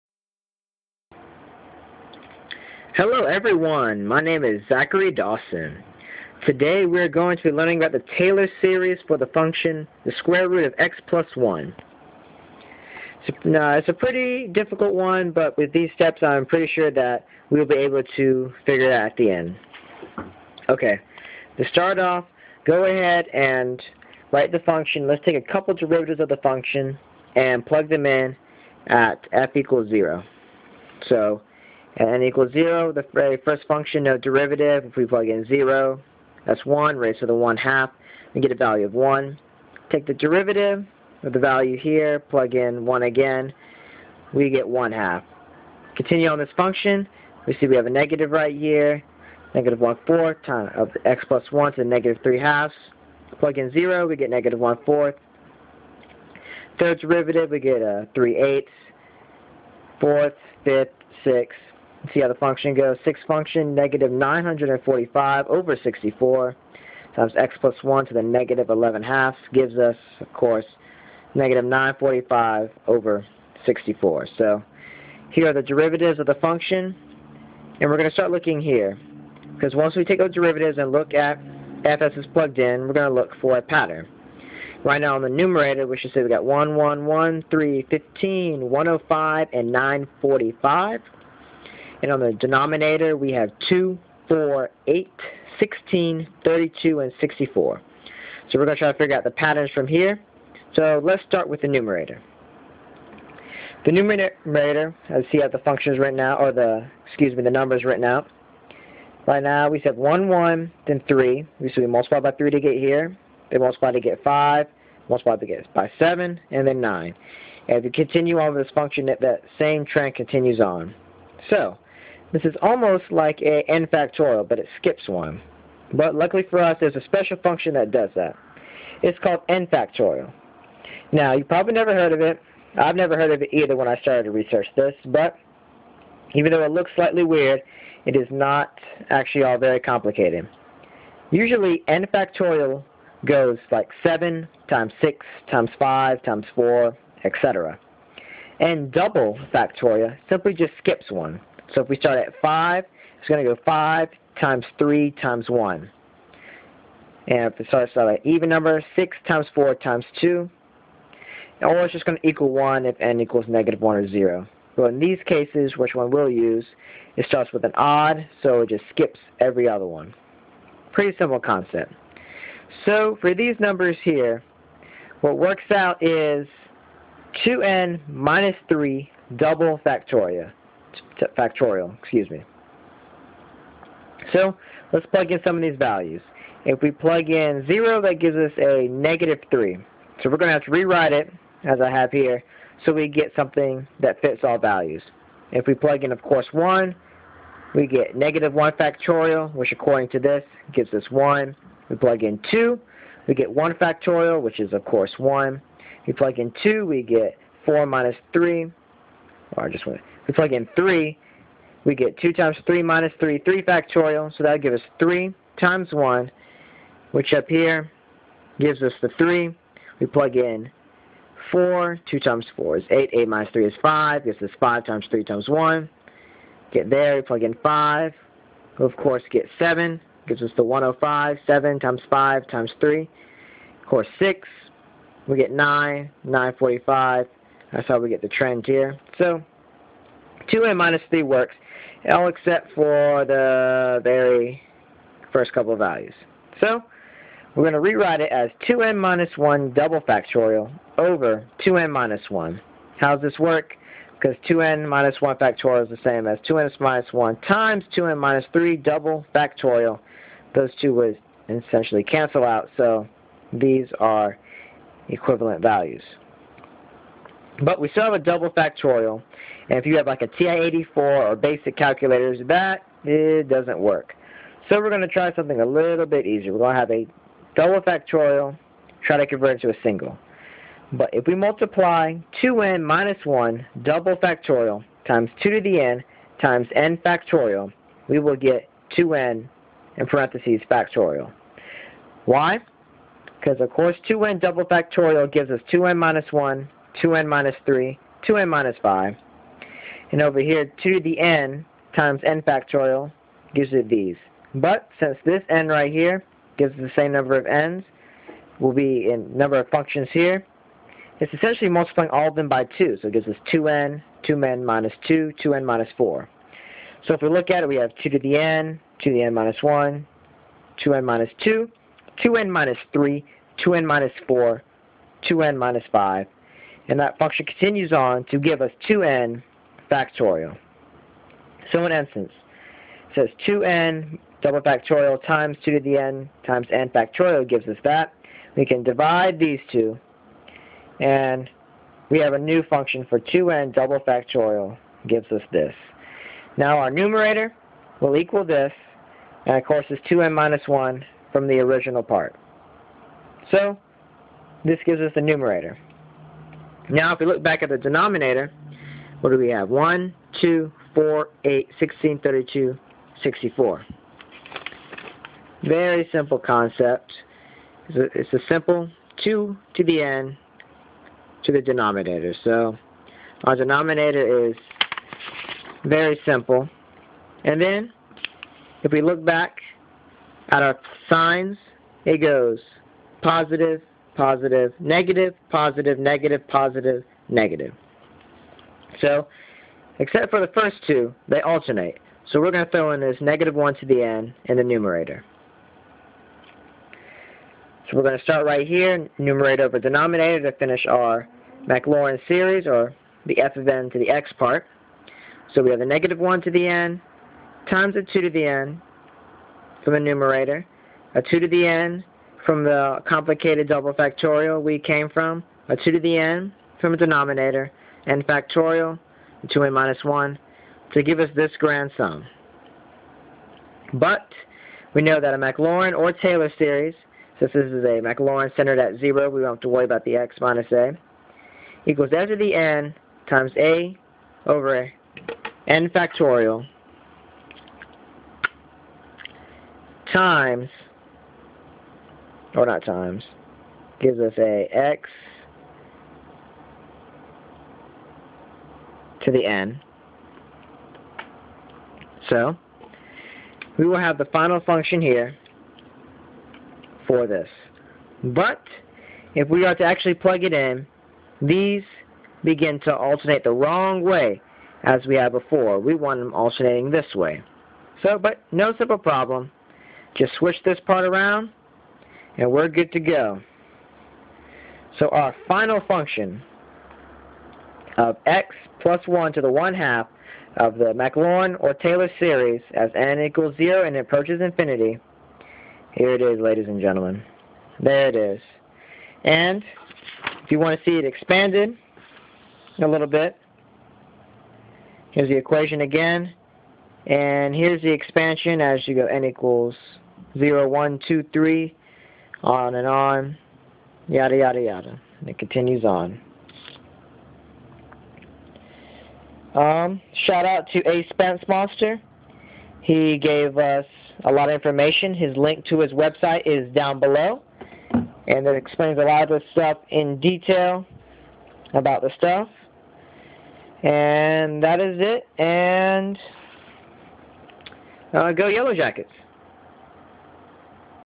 Taylor Series of sqrt(1+x)
This is the explanation of the expansion of the series sqrt(1+x) centered at (a) = 0.

S/O to aspensmonster. His website helped a great deal in explaining this function.

Go Yellow Jackets!

TI Calculator to aid in calculations